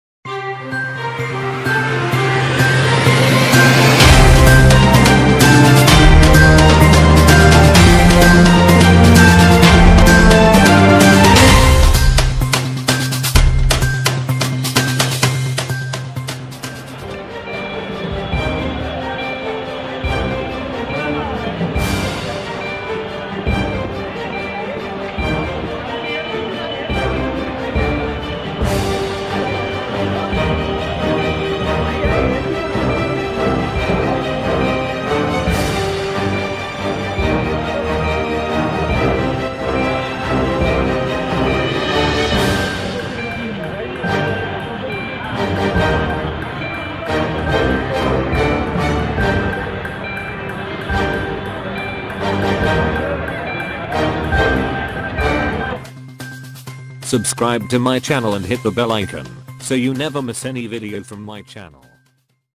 Bandra ke mtnl daftar me lagi aag
Mumbai ke bandra mtnl daftar me aaj achanak se afratafri jab faili tab wahan aag lagne ki khabar logo ko mili karib 100 se bhi zada log is mtnl ki imarat me the jab aag lagi thi kaaran pata nahi chal paya hai lekin aag karib 3 se 4 manzile tak ja chuki thi spot par police aur fire fighter team ki karib 13 gadiyo ne aag ko kabu me liya